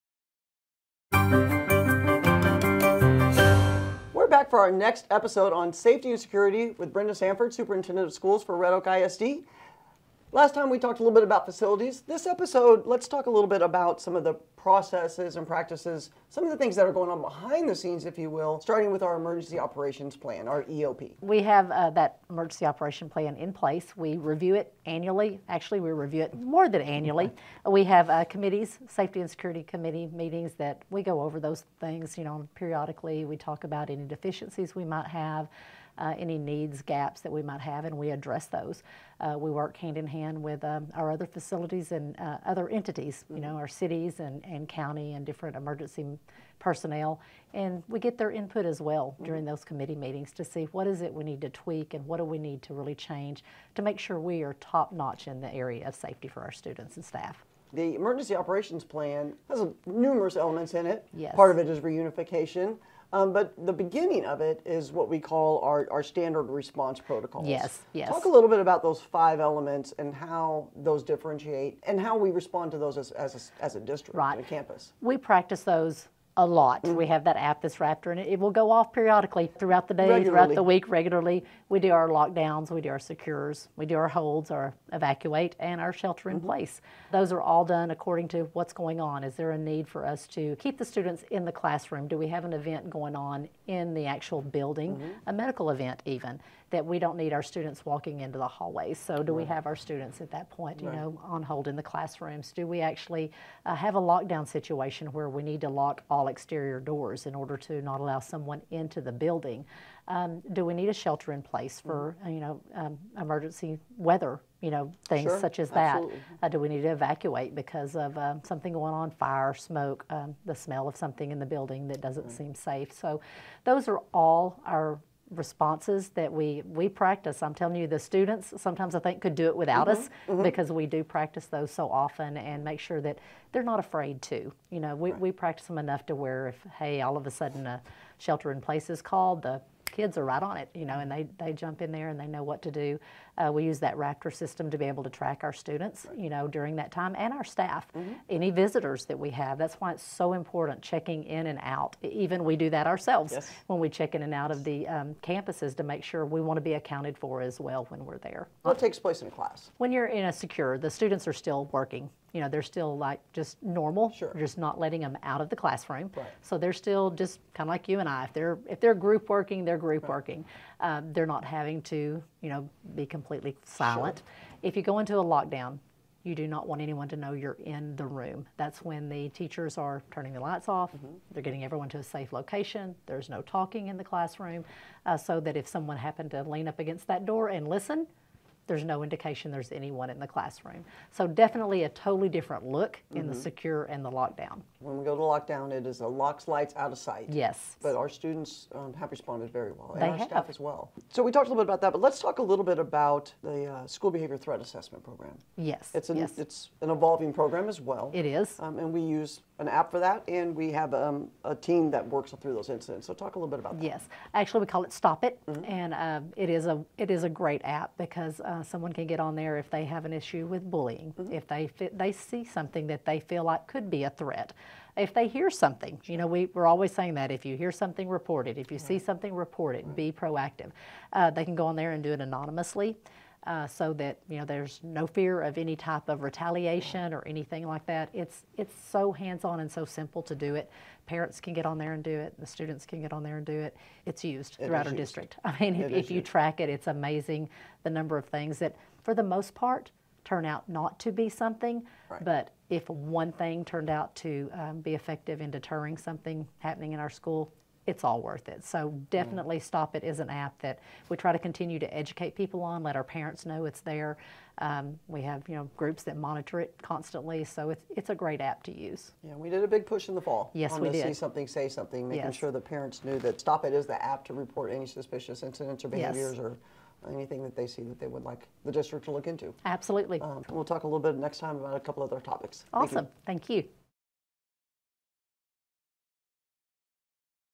Briefs With Brenda: Safety & Security Procedures
Join Superintendent Brenda Sanford in this edition of "Briefs With Brenda" as she discusses Red Oak ISD's commitment to student and staff safety. Discover the district's safety procedures, including emergency operations plans, standard response protocols, the Raptor system, and more, in this engaging conversation about prioritizing safety in education.